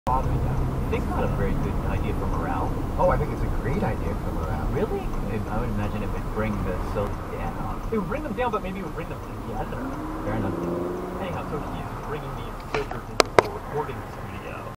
Redlight Runner Part 3
This is, like, the third person I have caught blatantly running a red light.